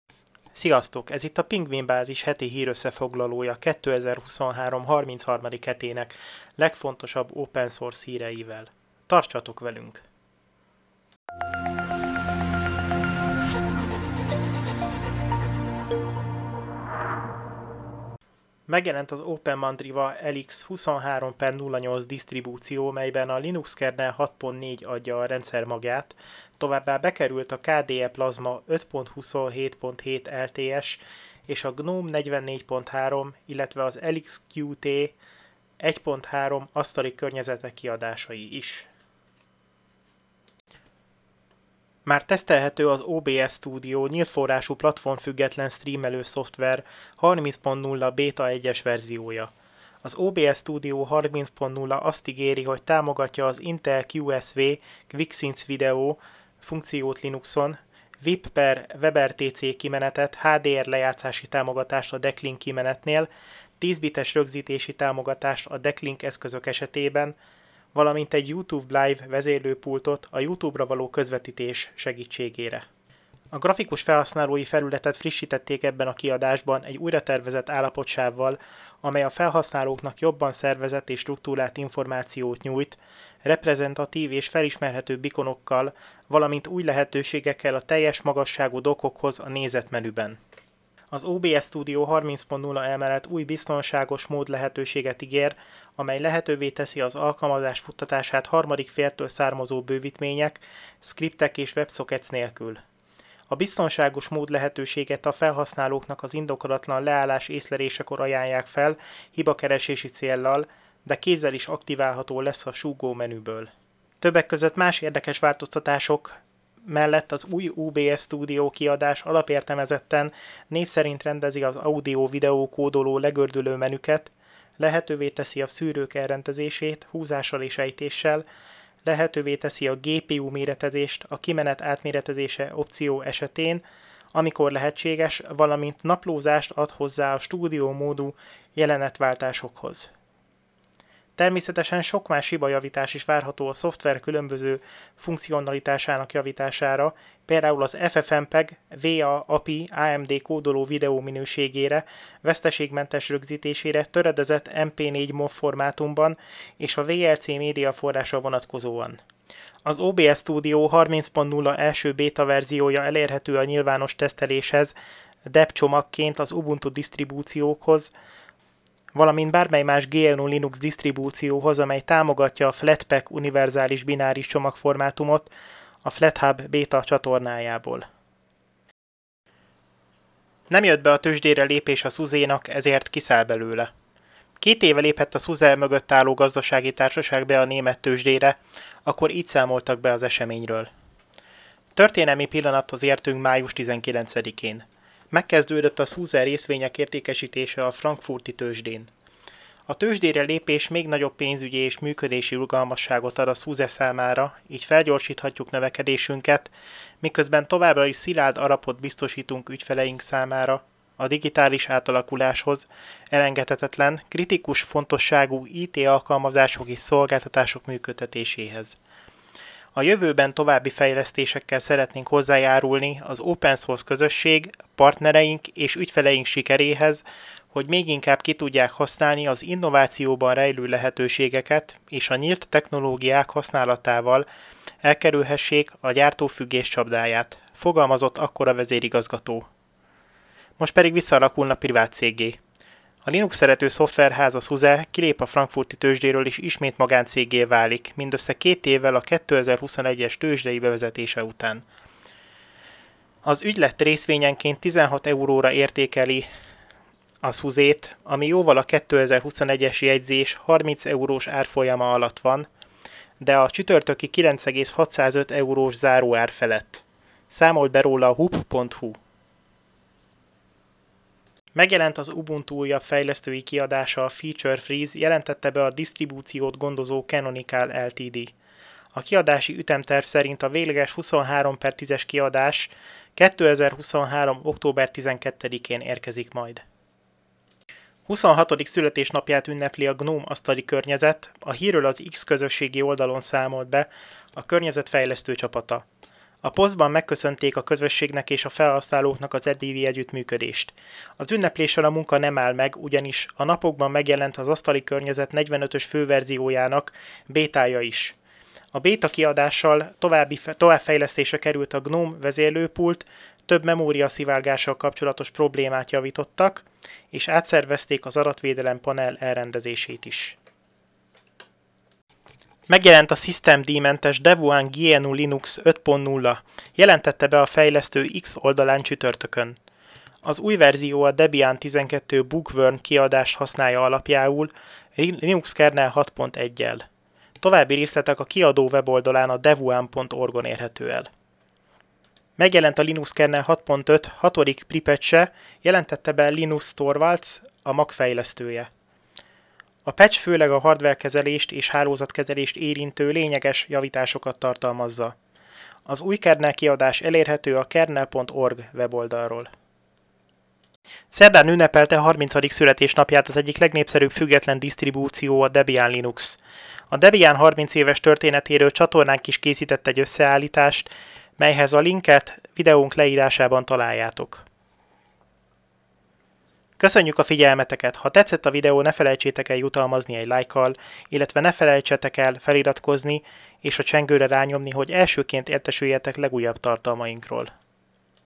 Heti hírösszefoglaló a szabad szoftverek világából 2023/33.hét
Heti hírösszefoglalónk tartalmából:
0:00 Előszó
0:18 Megjelent az OpenMandriva LX 23.08
0:40 Tesztelhető az OBS Studio 30.0beta1
3:00 Visszalép a német Tőzsdéről a SuSe
4:33 Megjelent az Ubuntu "Feature Freeze"
4:48 26 éves a GNOME, megjelent a 45-ös verzió bétája
5:22 Megjelent a Devuan 5.0
5:41 Megjelent a Linux kernel 6.5-rc6
6:00 30 éves lett a Debian/GNU Linux
6:16 Végszó